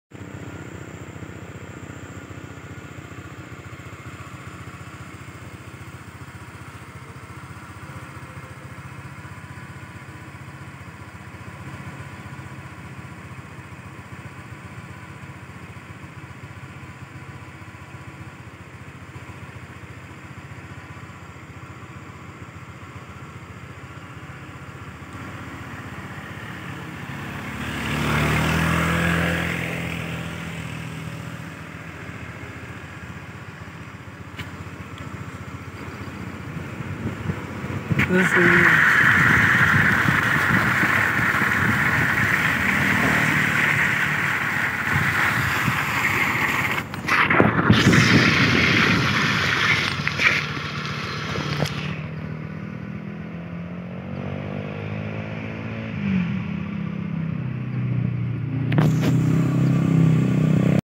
Jalan Desa Pujon Ngroto Torong Rejo
Jalan Desa Torong Yang Indah dan Naik sekali,masih sepi
Perjalanan yang bagus